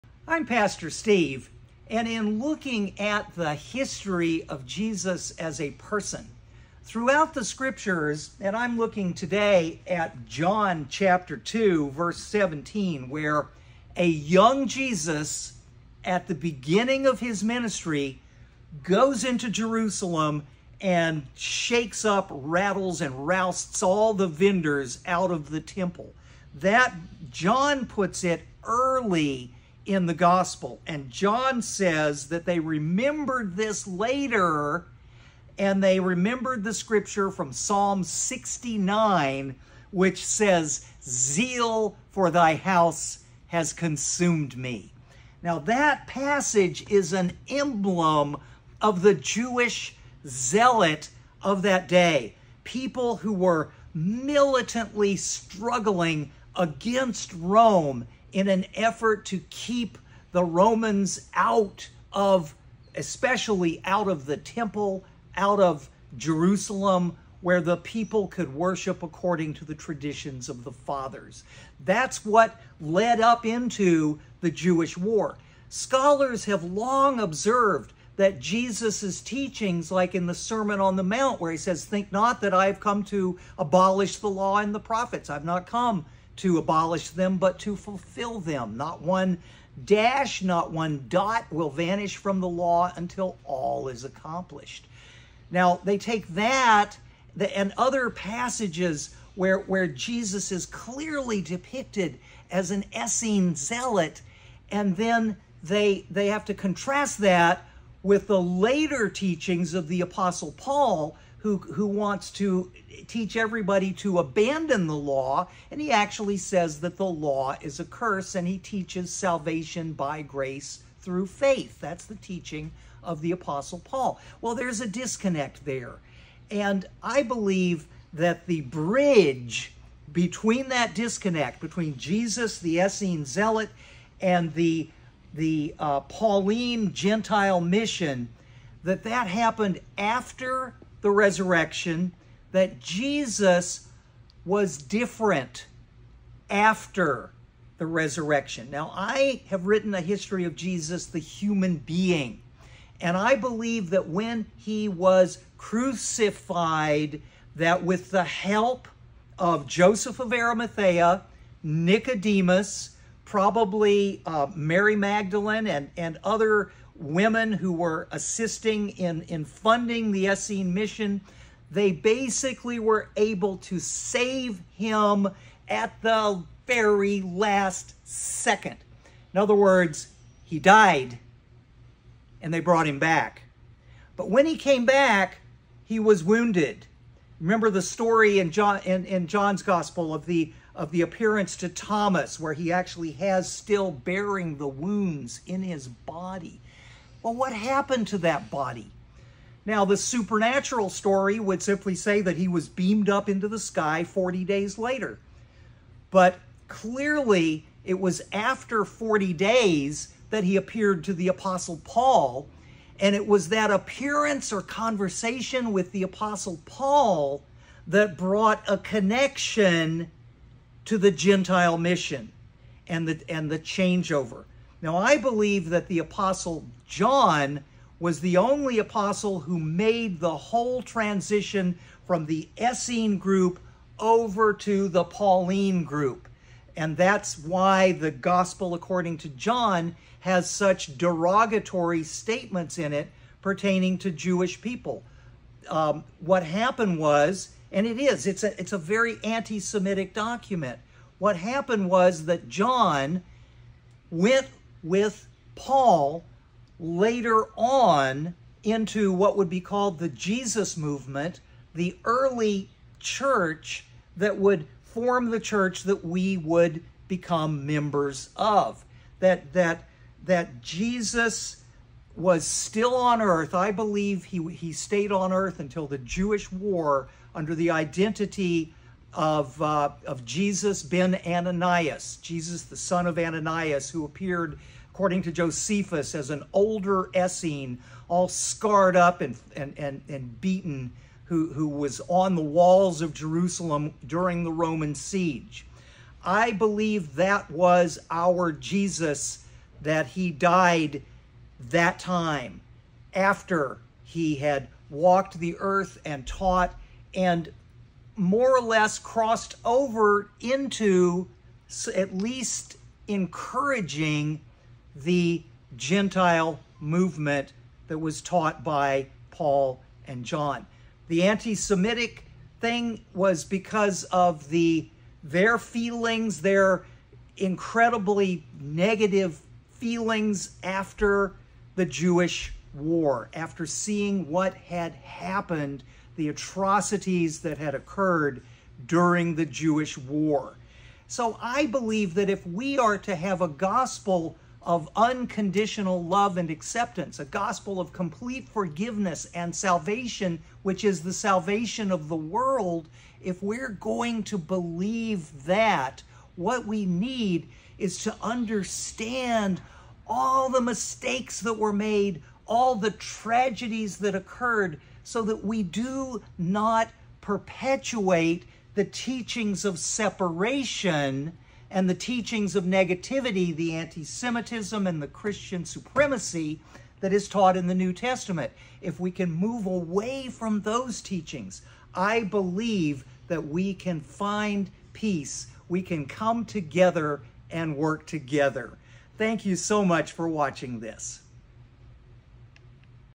The History of Jesus the Person by Pastor Steve
As a historical figure, Jesus was a Jewish man who lived in the Roman province of Judea during the 1st century. He was born in Bethlehem, a town near Jerusalem, and grew up in Nazareth, a small village in Galilee. Jesus' parents were Mary and Joseph, and he had several siblings, including James and Jude, according to the Gospels.

Jesus began his public ministry when he was baptized by John the Baptist in the Jordan River, and he spent the next few years traveling throughout Galilee and Judea, preaching about the coming of God's kingdom and performing miracles. He attracted a large following, including fishermen, tax collectors, and other ordinary people, as well as some prominent figures such as Mary Magdalene and Joseph of Arimathea.

Jesus' teachings emphasized love, forgiveness, and compassion, and he frequently challenged the religious and political authorities of his time, including the Pharisees, Sadducees, and Roman officials. He taught that all people were equal in the eyes of God, regardless of their social status or background.

Jesus was eventually arrested by the Roman authorities and sentenced to death by crucifixion, a common form of execution in the Roman Empire. He died on a cross outside of Jerusalem, but his followers claimed that he had risen from the dead three days later and had appeared to them. This event, known as the Resurrection, became the central belief of Christianity and led to the spread of the religion throughout the Roman Empire and beyond.

Overall, Jesus the historical figure was a charismatic religious leader who challenged the norms and beliefs of his time and left a lasting impact on history through his teachings and actions.